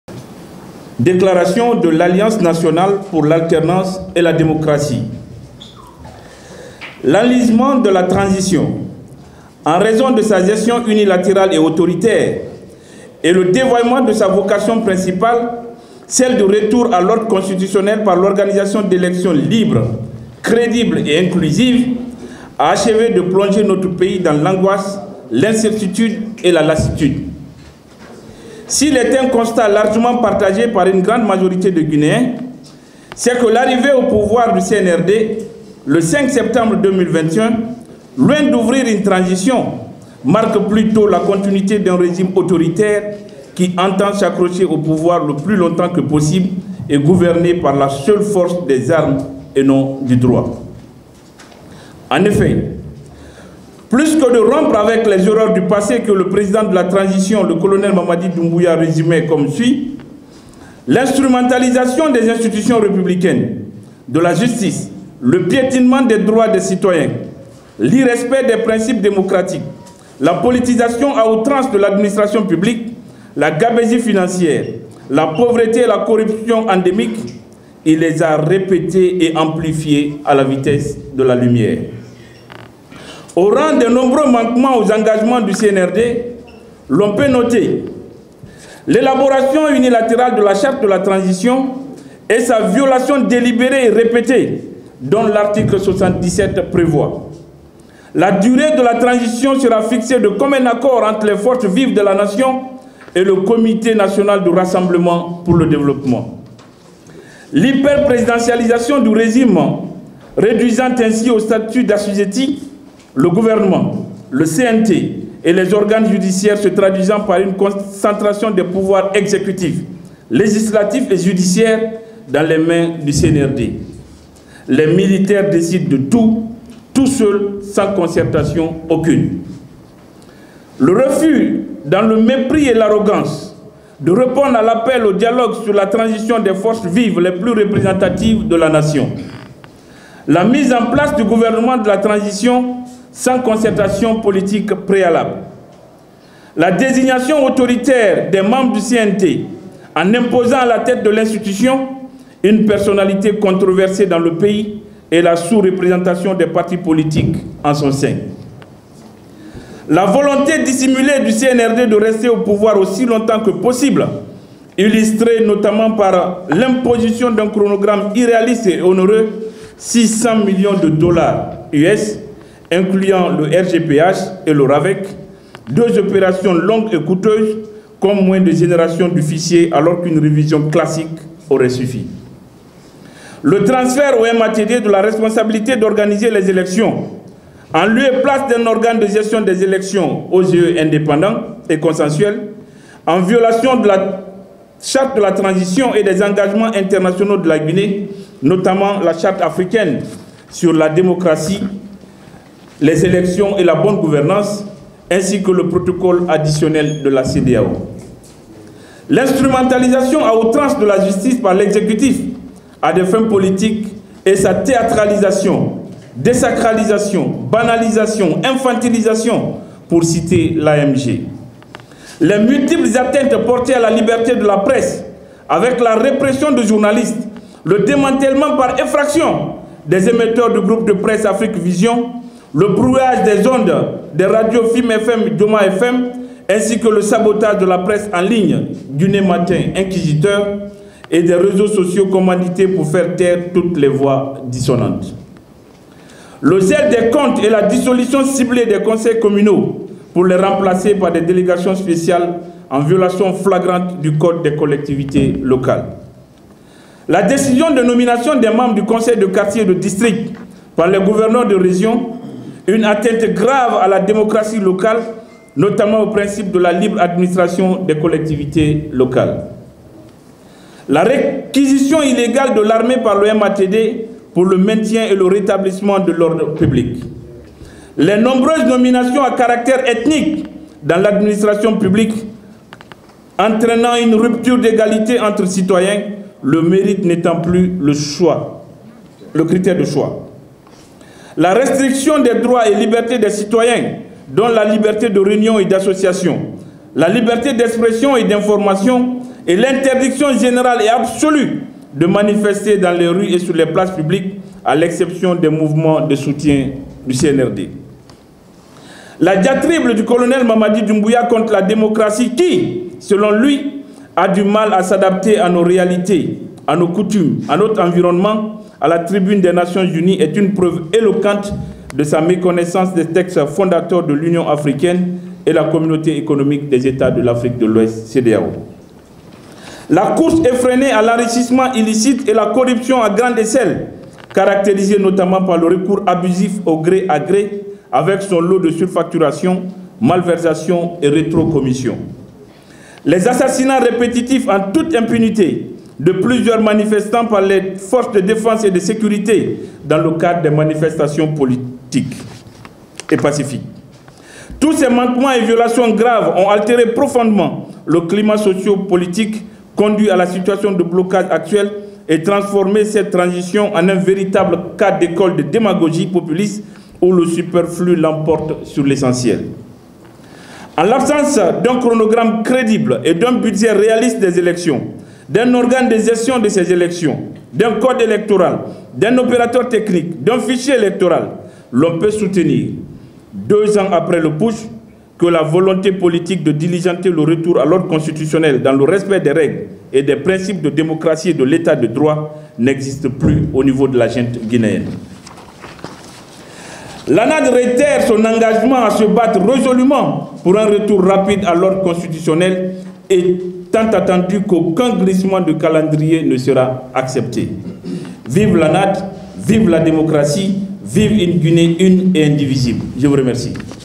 Transition: l’ANAD accuse le Colonel Mamadi Doumbouya d’avoir amplifié les maux qui minent la Guinée
L’Alliance nationale pour l’alternance et de la démocratie (ANAD) était en conférence de presse ce mercredi, 27 novembre 2023, à Conakry. Et, dans une déclaration lue par son responsable de communication, Souleymane Souza Konaté, cette structure dirigée par Cellou Dalein Diallo a dénoncé « l’enlisement de la Transition » guinéenne. L’ANAD a aussi accusé le président de la Transition, Colonel Mamadi Doumbouya, d’avoir « répétée et amplifié » les maux qui assaillent la Guinée depuis des années. Des maux qu’il avait pourtant promis de ne perpétuer, rapporte Guineematin.com à travers un de ses journalistes.

A sa prise de pouvoir le 5 septembre 2021, le colonel Mamadi Doumbouya s’était engagé à lutter contre les délits financier (corruption, détournement de deniers publics…) et tous les travers sociopolitiques et administratives qui entrave le développement de la Guinée. Mais, pour l’ANAD, le président de la Transition a dévié de ce chemin honorable pour perpétuer et amplifier ces pratiques qu’il avait promis de combattre.

« L’enlisement de la transition, en raison de sa gestion unilatérale et autoritaire, et le dévoiement de sa vocation principale, celle du retour à l’ordre constitutionnel par l’organisation d’élections libres et crédibles, a achevé de plonger notre pays dans l’angoisse, l’incertitude et la lassitude. S’il est un constat largement partagé par une grande majorité de Guinéens, c’est que l’arrivée au pouvoir du CNRD, le 5 septembre 2021, loin d’ouvrir une transition, marque plutôt la continuité d’un régime autoritaire qui entend s’accrocher au pouvoir aussi longtemps que possible et gouverner par la seule force des armes et non du droit. En effet, plutôt que de rompre avec les erreurs du passé que le Président de la transition, Mamadi Doumbouya, résumait comme suit : l’instrumentalisation des institutions républicaines, de la justice, le piétinement des droits des citoyens, l’irrespect des principes démocratiques, la politisation à outrance de l’administration publique, la gabegie financière, la pauvreté et la corruption endémique..., il les a répétées et amplifiées à la vitesse de la lumière », a déclaré l’ANAD.